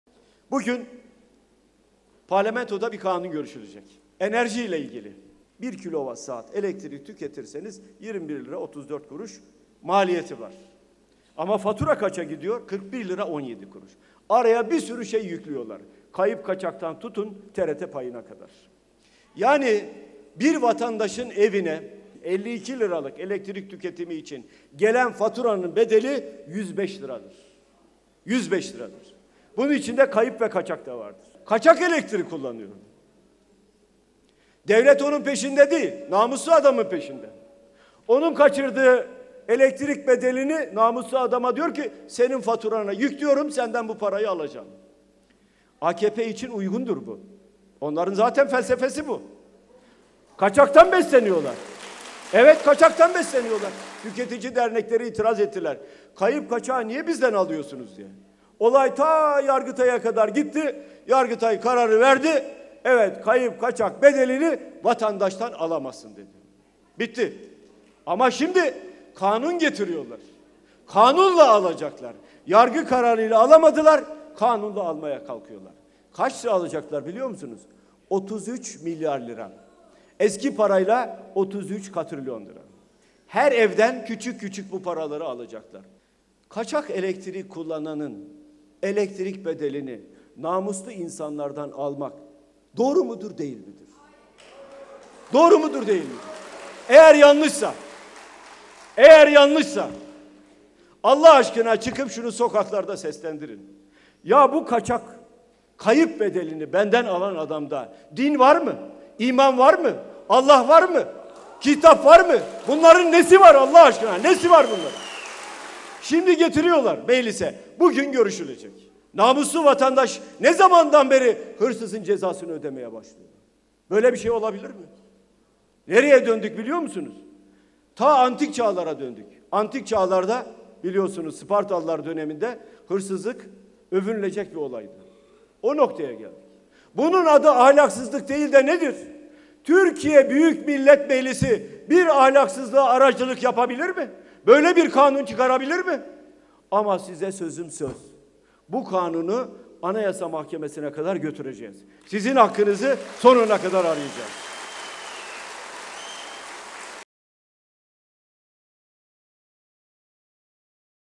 Elektrik Soygununu AYM'ye Kadar Götüreceğiz!
Yargıtay 'vatandaştan kayıp kaçak bedelini alamazsın' dedi. Onlar bugün, bu bedel alınsın diye kanun çıkarmaya hazırlanıyorlar. Her evden küçük küçük alacaklar bu parayı! Kaçak elektriğin bedelini namuslu vatandaşlardan almanın nedeni nedir?